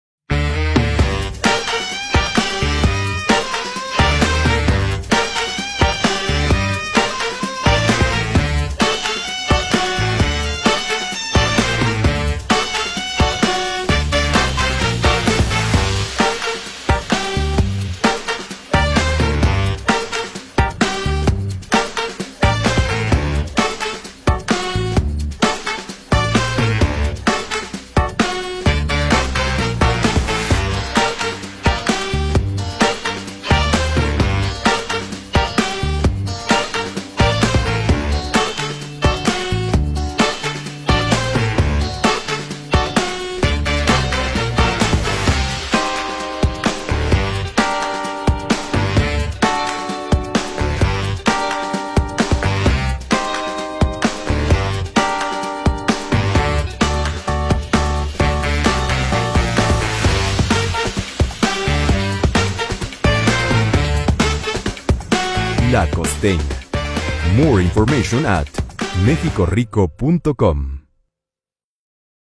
Chicken Alambre Taco
Ingredients:
• 1 Chicken breast
• 1 lime
• 1 tsp Salt
• 1 tsp Pepper
• 1 lb. Bacon
• Bell pepper
• ½ Onion
• La Costeña Refried Pinto Beans
• La Costeña Jalapeños
• Mozzarella cheese

Directions:
Fry bacon.
In a bowl marinate chicken with salt, pepper and lime juice
On a skillet over medium high heat, cook chicken.
When chicken is cooked add the bacon and mix.
Add the onion and bell peppers.
Cook for about 5 more minutes.
Add mozzarella cheese on top.
Serve when cheese completely melted.
Complement with a side of La Costeña Refried Pinto Beans.